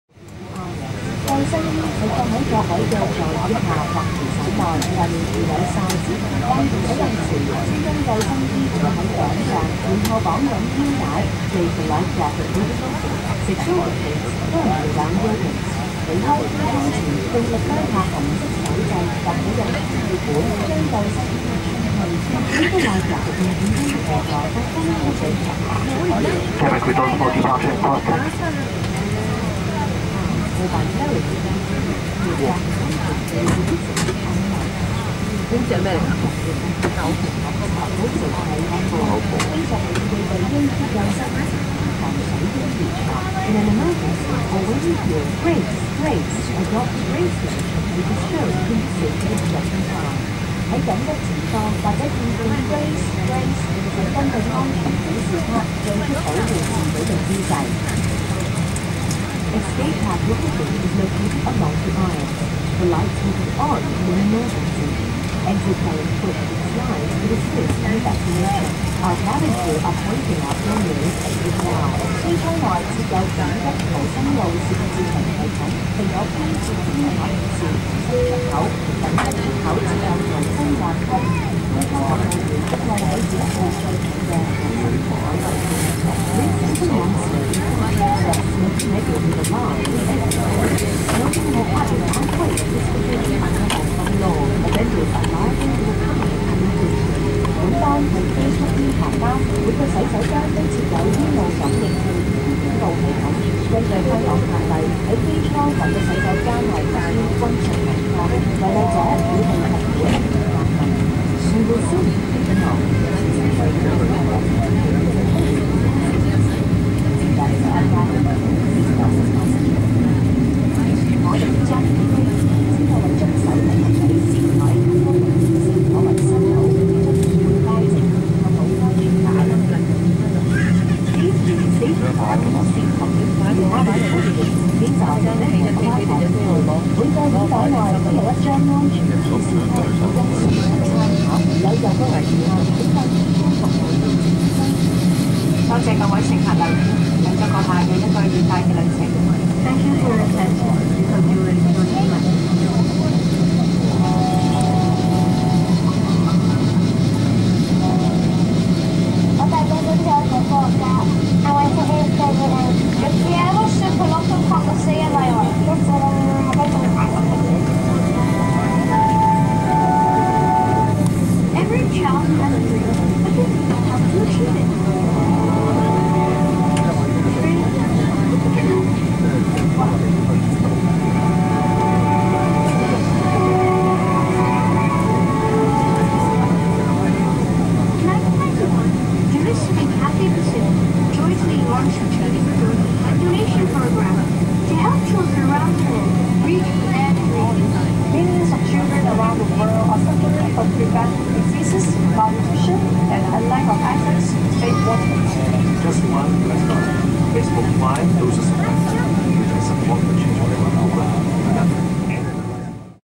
Cathay Pacific Boeing 777-267 Pushback from Hong Kong-HKG (CX633)
Aboard Cathay Pacific Airways flight CX633 pushing back from the gate in Hong Kong Intl. Airport bound for Penang, Malaysia.
Video includes pushback and startup.

Airline -- Cathay Pacific Airways (CX / CPA)
Flight -- CX 633 (HKG - PEN)
Aircraft -- Boeing 777-267
Registration -- B-HNA
Seat -- 55A (Economy)
Departed -- 08:25AM HKT - Gate 1 - Runway 07R
Arrived -- 12:05PM MYT - Gate A6 - Runway 04